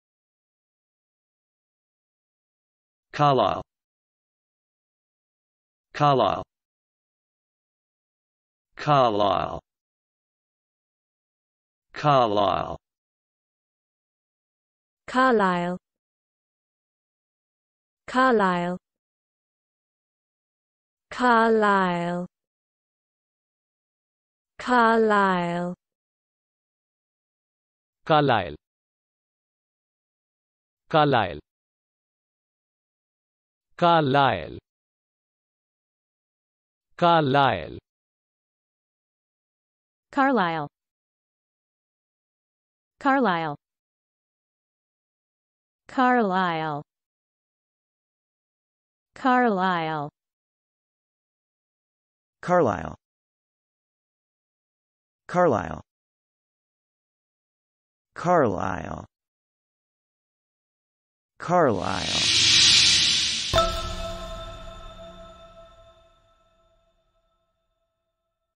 How to Pronounce "Carlisle"
Trying to learn English?

Welcome to the video on how to correctly say "Carlisle".

Oops!! We don't have a definition for "Carlisle", can you give us a hand? Let's discuss Carlisle in the comments.

Fancy submitting your own word?